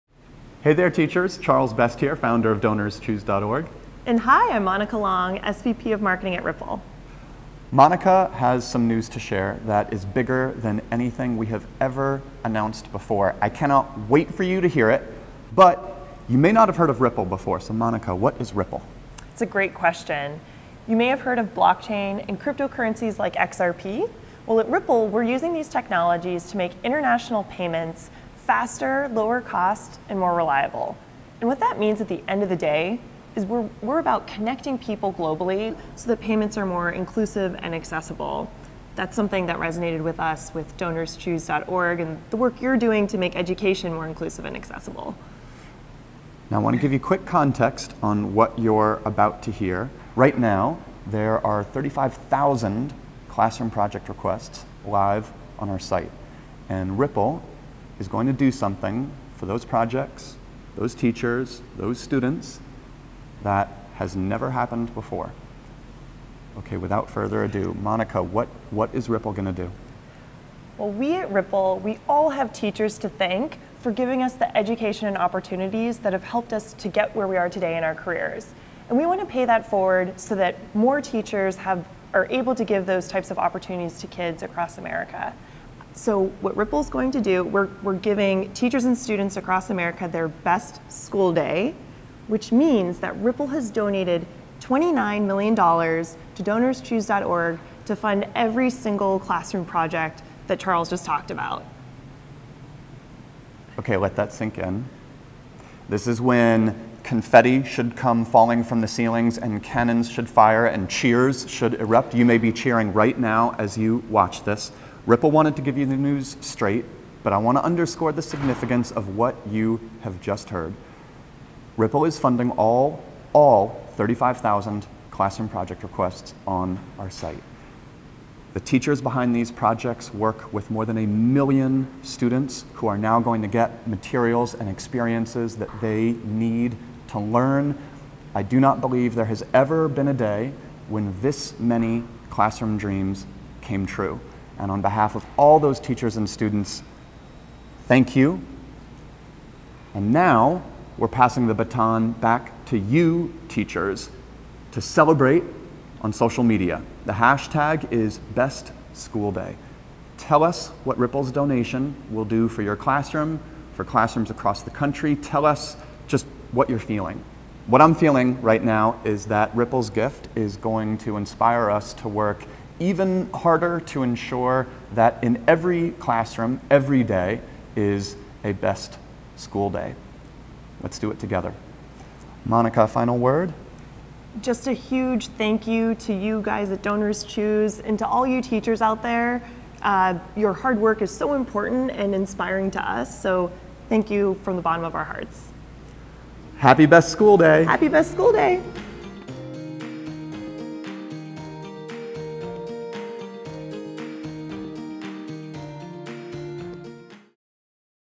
BestSchoolDay 2018: Every Project Funded!
With the largest single donation in our history, Ripple funded EVERY SINGLE CLASSROOM PROJECT on DonorsChoose.org.